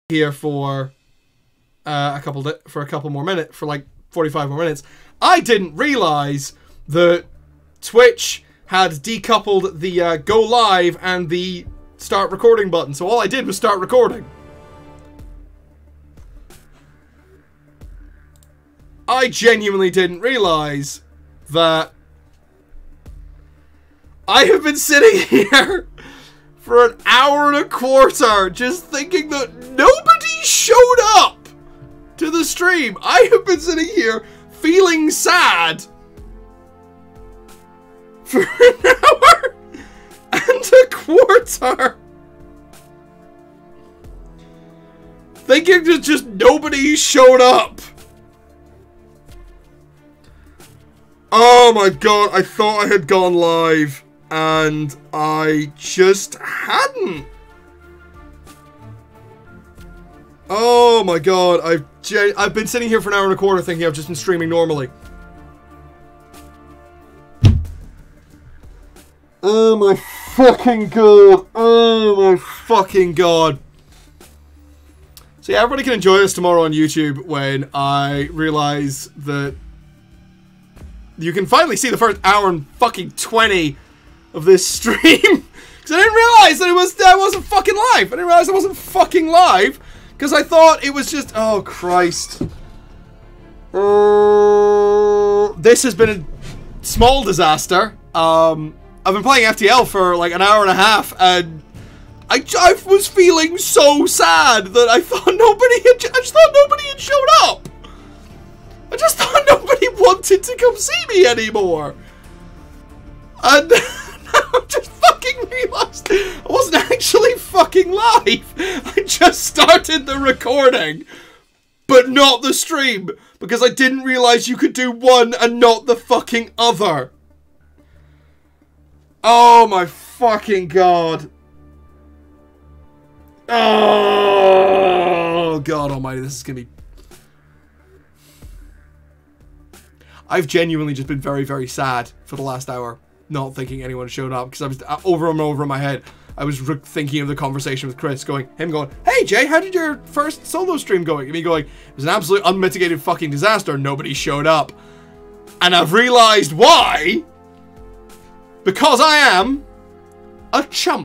Complete lemon realises that he hasn't been streaming this whole time
If you look closely, you can see the exact moment that his brain explodes from embarrassment